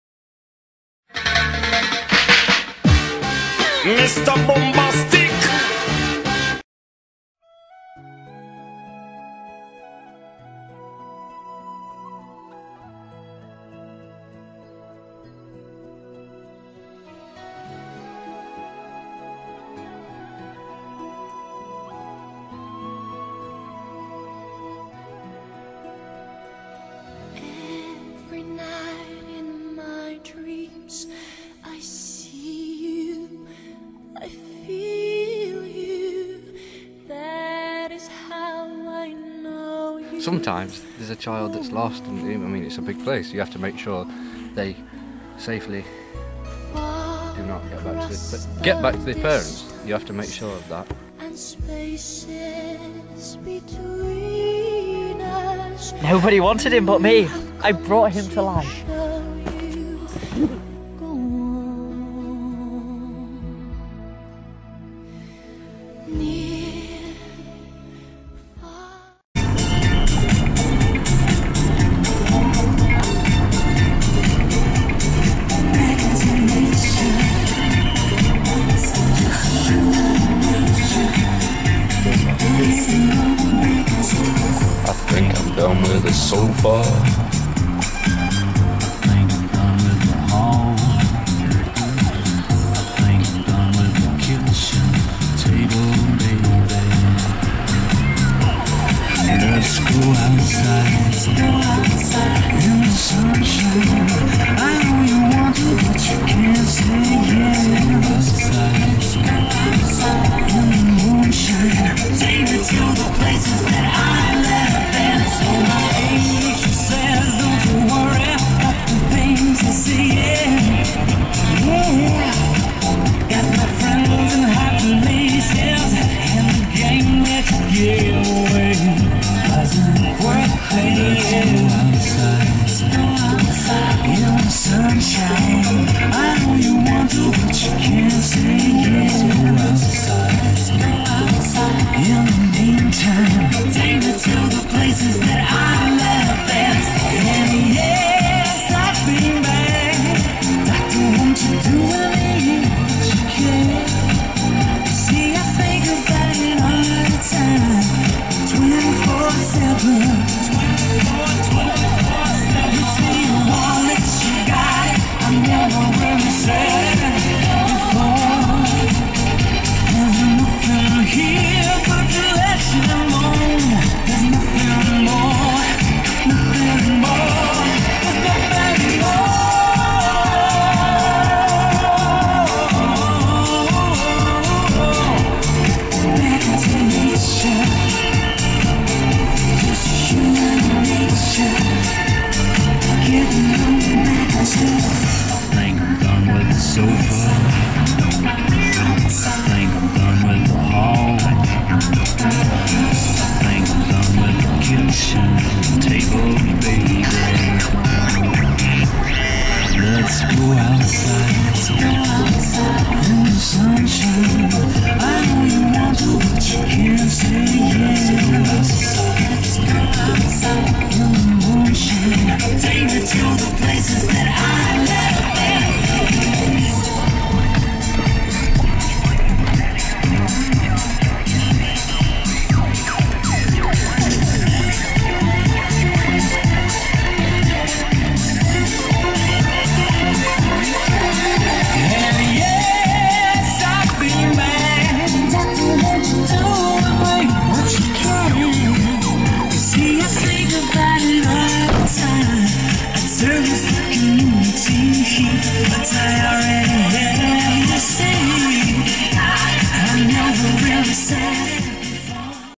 Jed Jensen: my life - The Trailer
Beat103 Productions presents a new Comedy Documentary based on an old sketch, the mansfield radio sketch; Jed Jensen: My Life. Staring Kyle Ashmore as Jed Jensen and James Day as Eugene Briggs. Coming soon to Beat103Productions Channel.